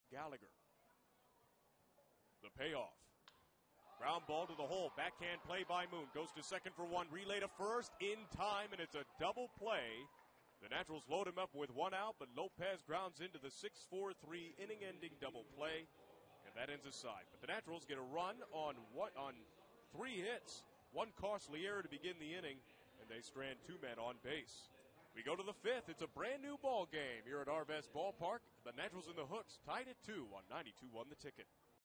Corpus Christi's Moon starts 6-4-3 double play
6/3/16: Corpus Christi's Chan Jong Moon starts a 6-4-3 double play with the bases loaded to end the fourth inning against Northwest Arkansas